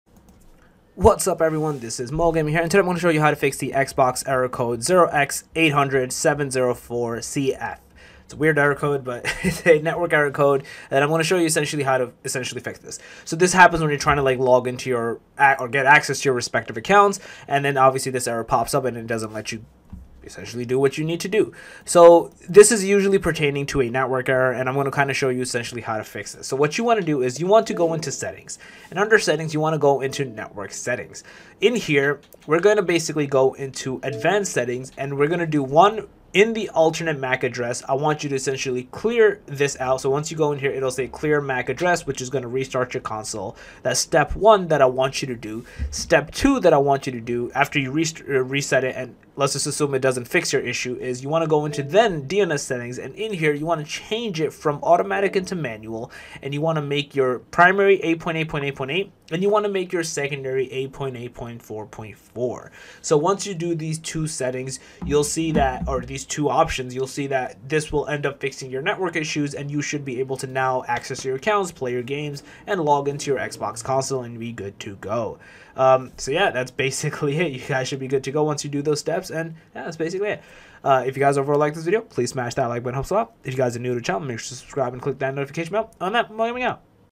How To Fix Xbox One / Series X/S Error Code 0x800704cf
In this video I talk about How To Fix Xbox One / Series X/S Error Code 0x800704cf